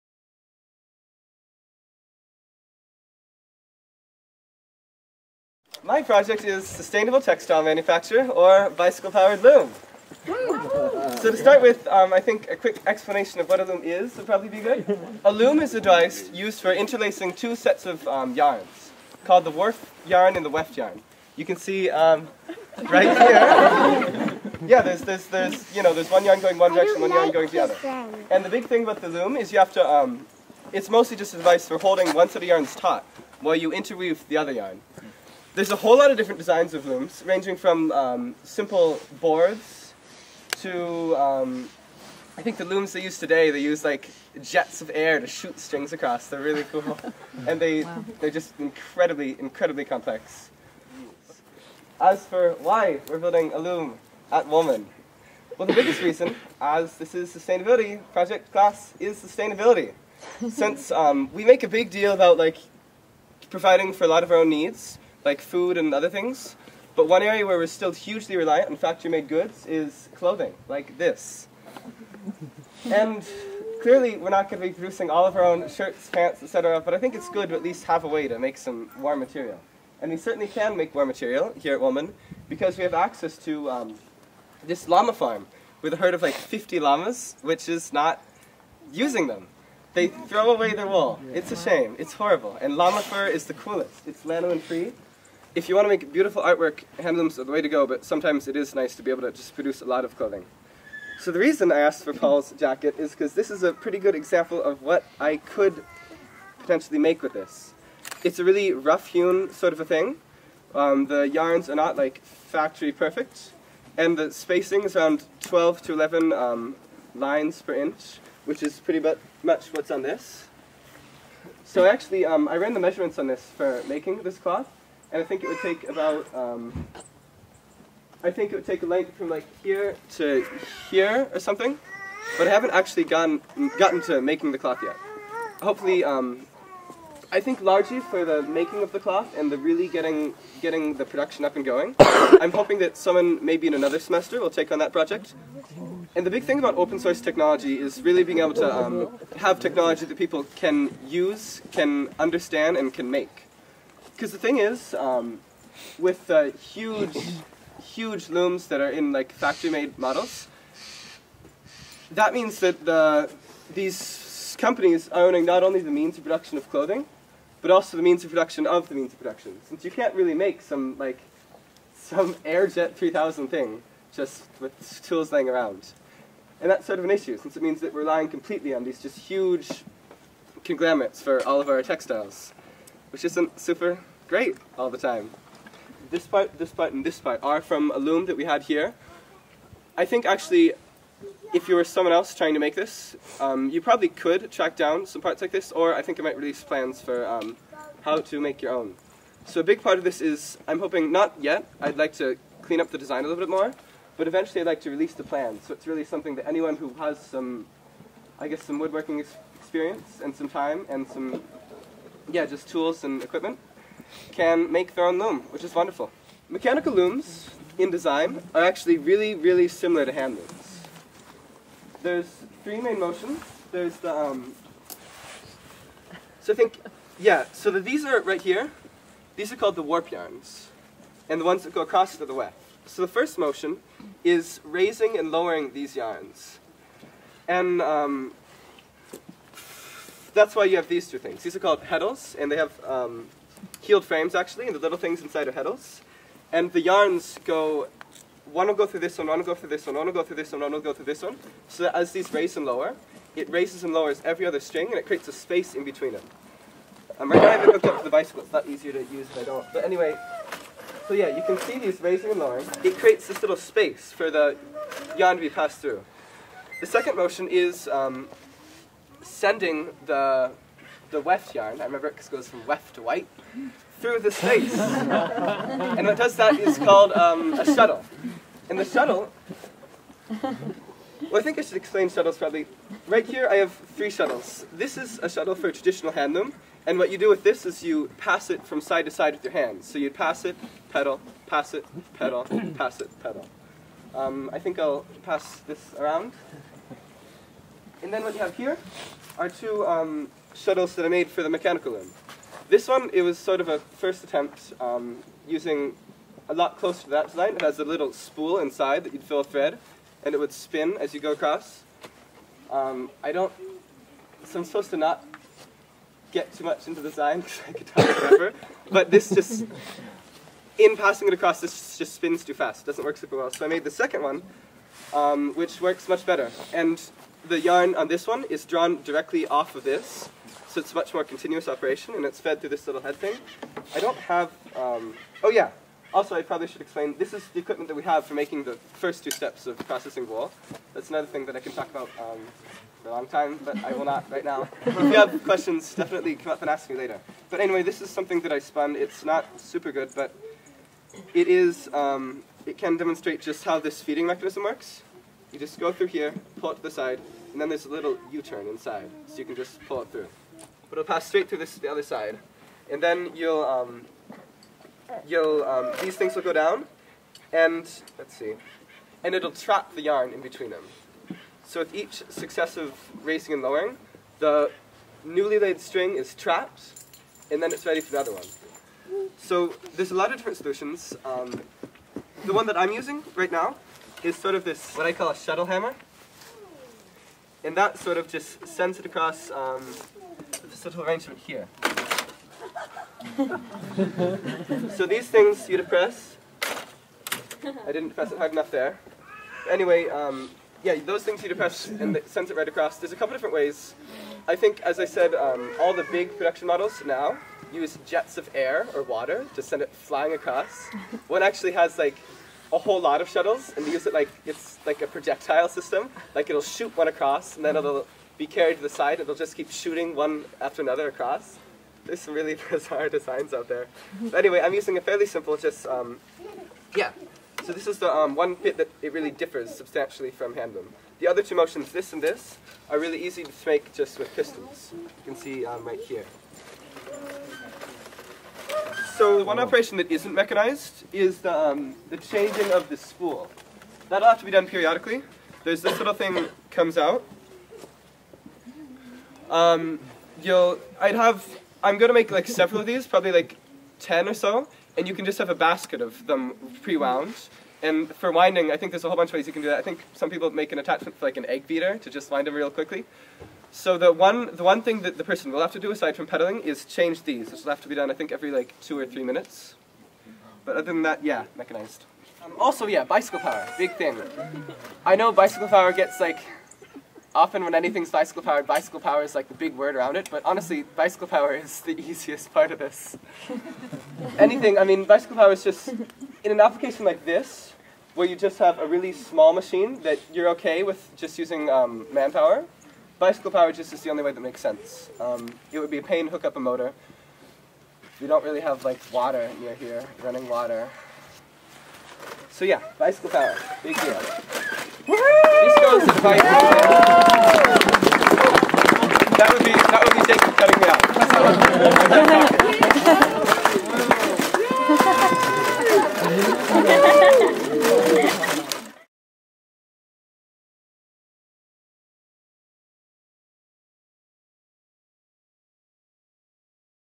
The Woolman Semester Bicycle Powered Loom
The Woolman Semester Student Russell Hofvendahl designs and builds a bicycle-powered loom for his Sustainability project.